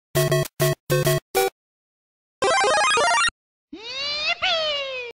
Super Mario Test
"YEEEEE-IPPY!"
Just a little sound / animation test with Mario eating a mushroom.
Tell me what you think about it in the comments.